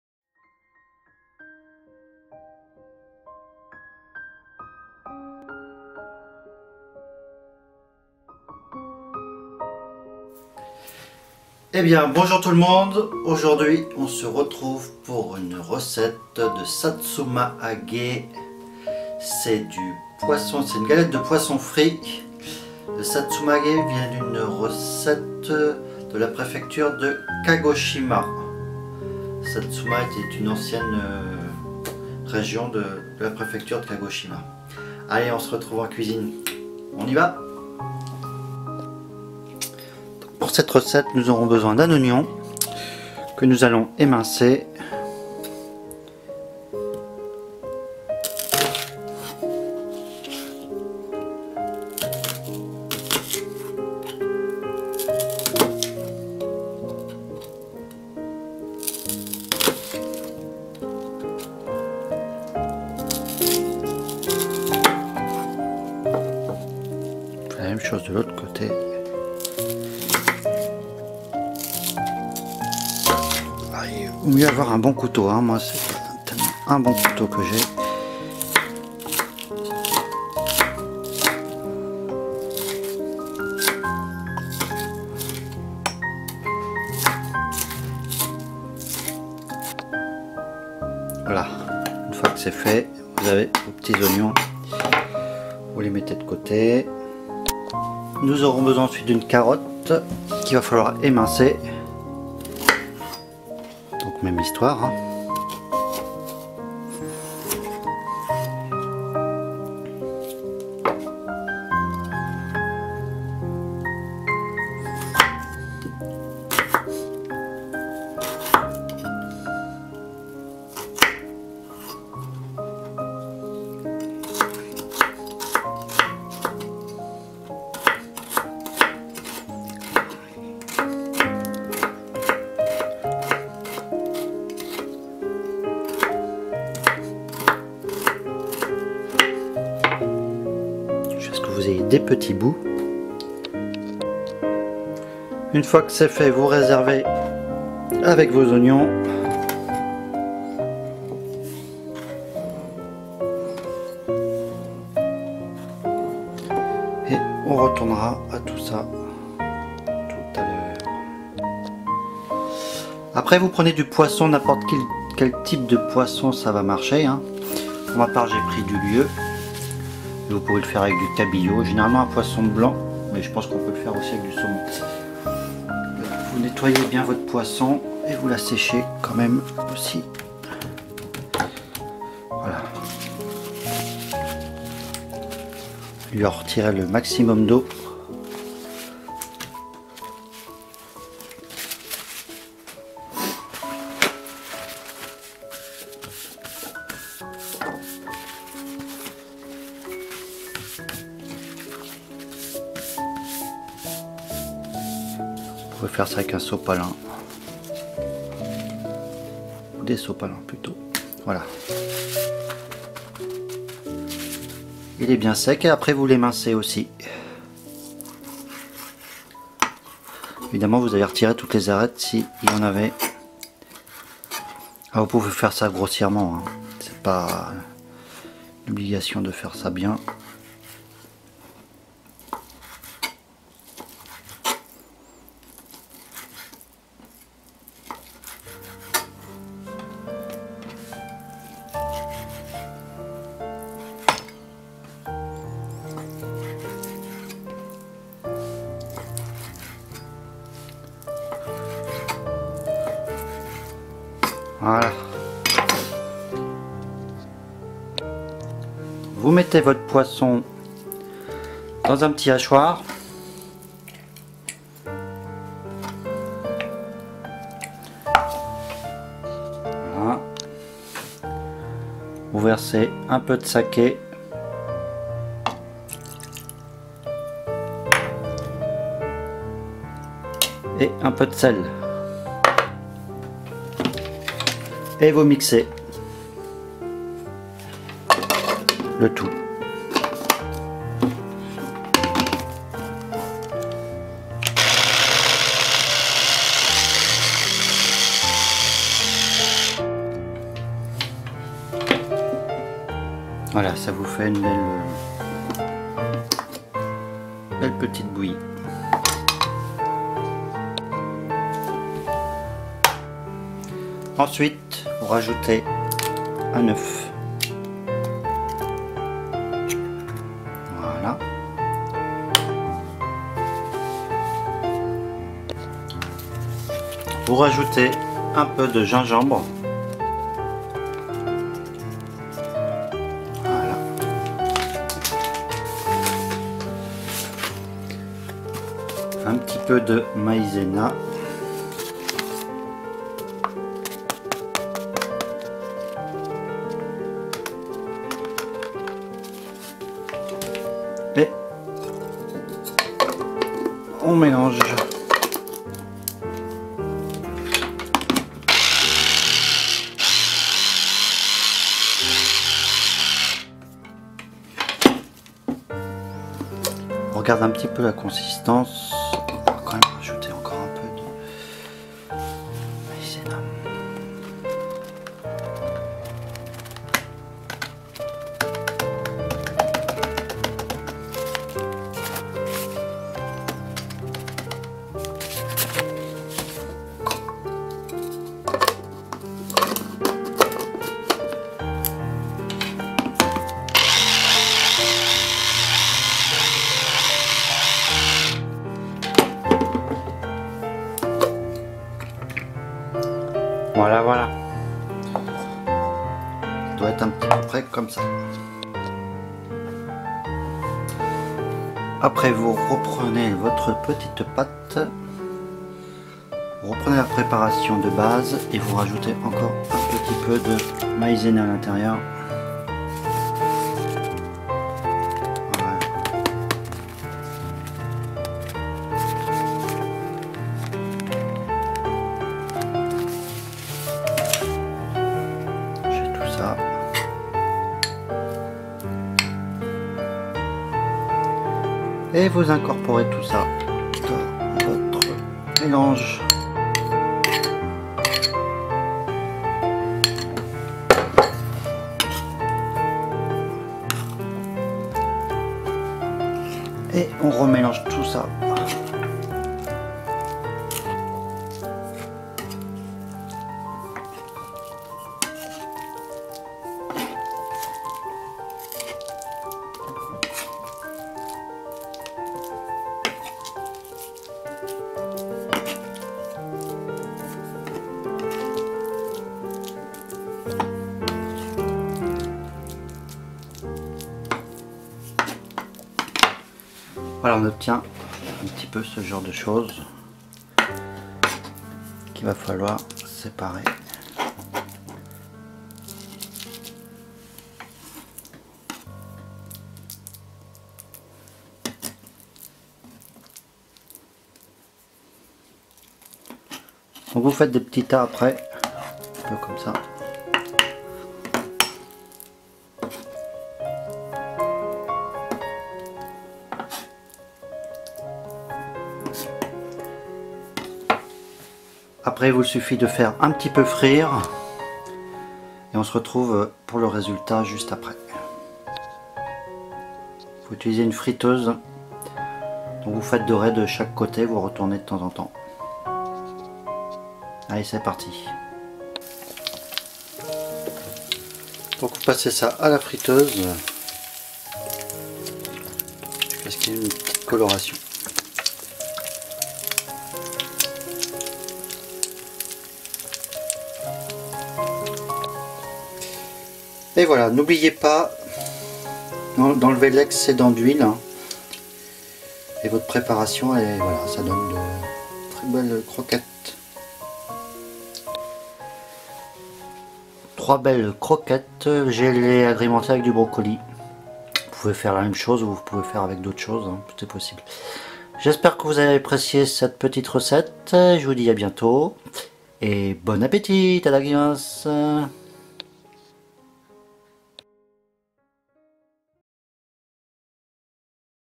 satsuma age
gateau de poisson fris à la mode kagoshima, à tester^^